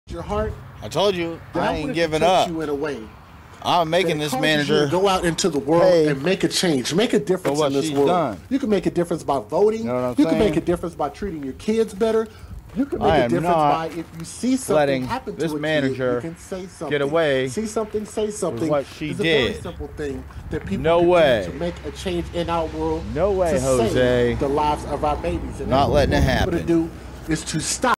5-11-20 Part 3 - JJ on His way! Demonic Descriminating Dollar General Manager Has Hell To Pay
On the side of the road part two, let's see where this goes!